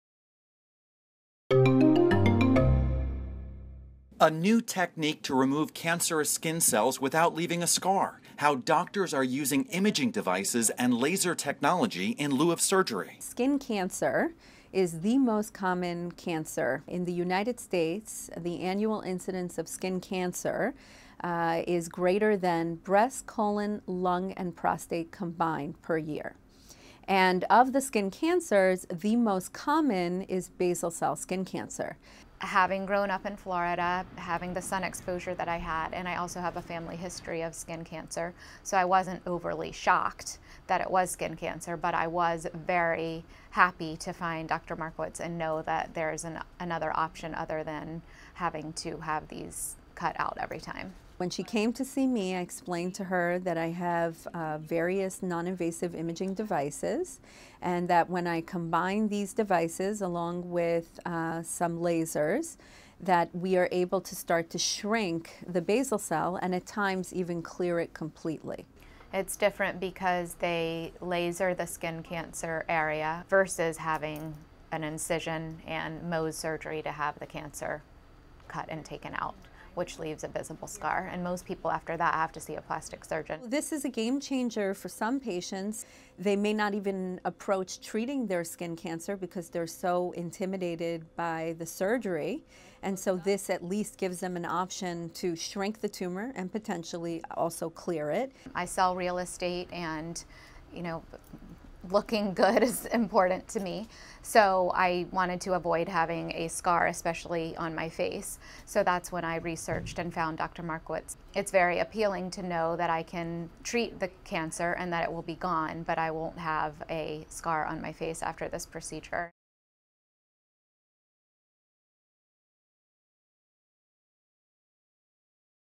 Mount Sinai Spotlight: Skin Cancer Removal
Dr. Orit Markowitz, MD, assistant professor of dermatology at the Icahn School of Medicine at Mount Sinai, discusses how she removes cancerous skin cells without leaving a scar. Dr. Markowitz uses image devices and laser technology in lieu of surgery.